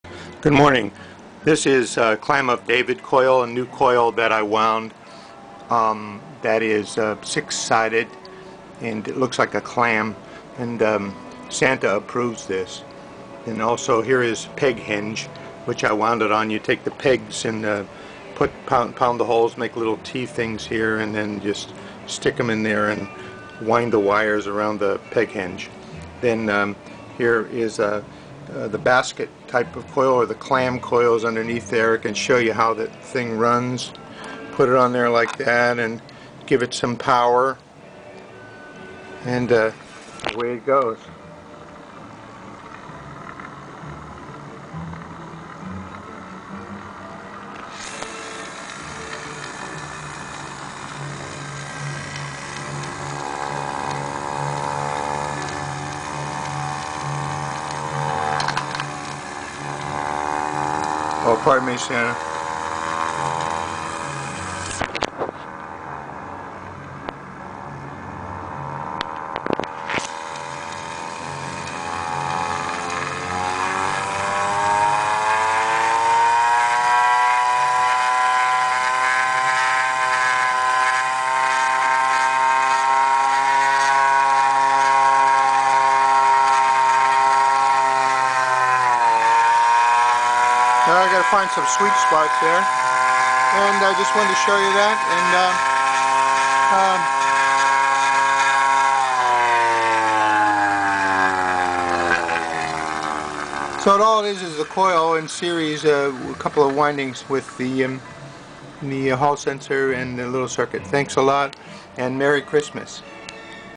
Clam of David Coil with Pulse Motor
Clam of David Coil Pulse Motor.   This coil is a starship variant of an Arturo Triadic Coil.   It is bifilar, about 50 turns of #24 magnet wire on each winding.   Used my Peghenge pegboard with T-ended pegs as a winding jig.    This is a powerful coil in my estimation.    Seems to work well with both winds in series, and with the connections reversed or in parallel also.   Still checking that out.  The neoroto can run mounted over the coil, unobstructed.  Nothing is in the bowl but the neorotor magnet and the drive shaft.            Saint Nicholas came by and approved the coil,
but his bag is already too full for inclusion in the sleigh run this year, maybe next year.       Thanks to hhoforvolts, lidmotor, jonnydavro, GBLuer, Russ G, and others for their videos this year also.        Checkout  vortexspace.org

Aloha and Merry Christmas.